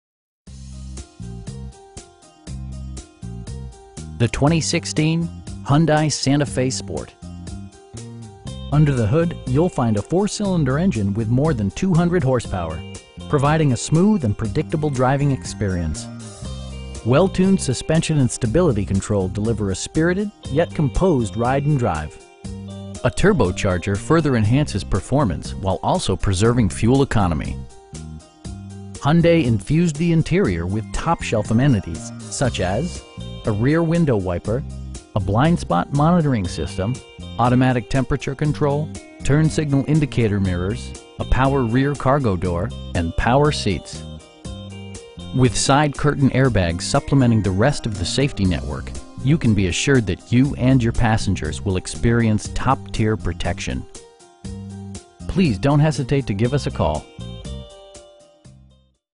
2016 Hyundai Santa Fe Sport in Lubbock, TX 79407
4025 W. Loop 289

The 2016 Hyundai Santa Fe Sport. Under the hood you'll find a 4 cylinder engine with more than 200 horsepower, providing a smooth and predictable driving experience. Well tuned suspension and stability control deliver a spirited, yet composed, ride and drive A turbocharger further enhances performance, while also preserving fuel economy. Hyundai infused the interior with top shelf amenities, such as: a rear window wiper, a blind spot monitoring system, automatic temperature control, turn signal indicator mirrors, a power rear cargo door, an overhead console, and power seats. With side curtain airbags supplementing the rest of the safety network, you can be assured that you and your passengers will experience top-tier protection. Our aim is to provide our customers with the best prices and service at all times. Please don't hesitate to give us a call.